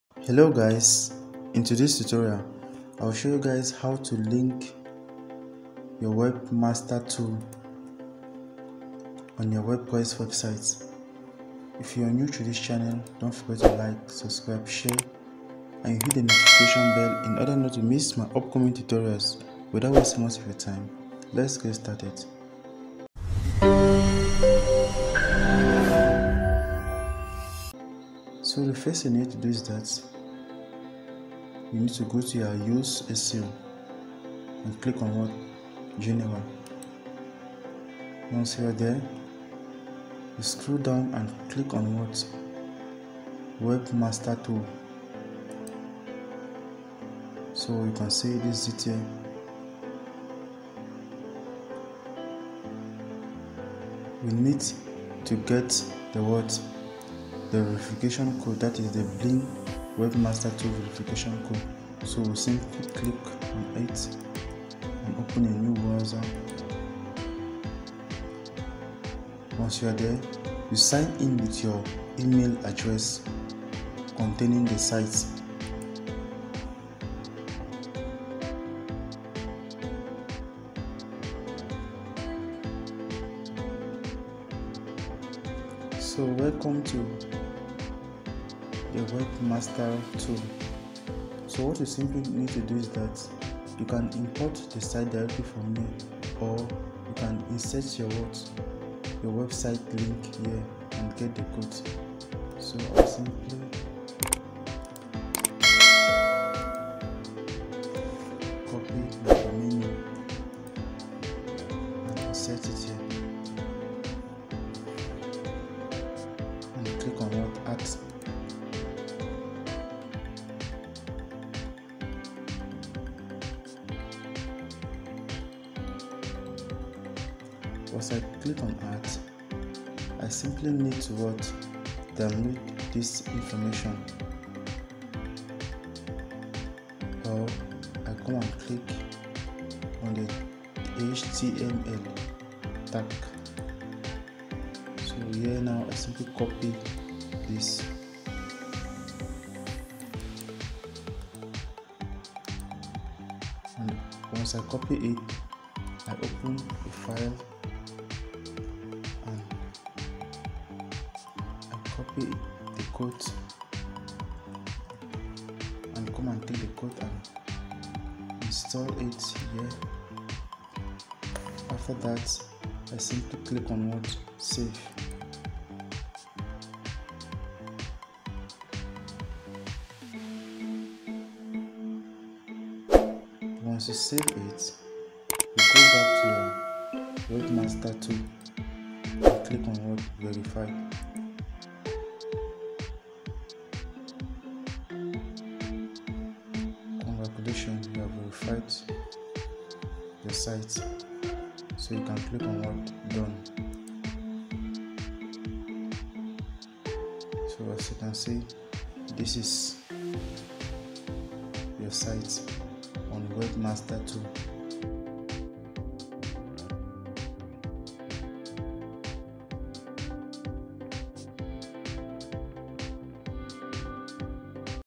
How to Add Website to Bing Search Engine || Bing Webmaster Tools
Hello friend  In this video, I show you how to submit your website to the Bing search engine. Follow along with me as I show you how to submit your website using Bing Webmaster Tools. First, we're going to verify your website using the Yoast plugin on WordPress, then we're going to submit your sitemap so that you can start getting indexed and found on Bing.

2️⃣  Sign in (or create a Bing user first)
3️⃣  Enter your website URL under “Add a Site”
4️⃣  Type in your full website URL including the “HTTP” or “HTTPS”
5️⃣  Verify using the “HTML tag” method
6️⃣  Add HTML meta tag to the website using Yoast plugin
7️⃣  Click “Verify”
8️⃣  Finally, navigate to “Sitemaps” in Bing Webmaster Tools
9️⃣  Enter and submit your sitemap

Elec tv Tags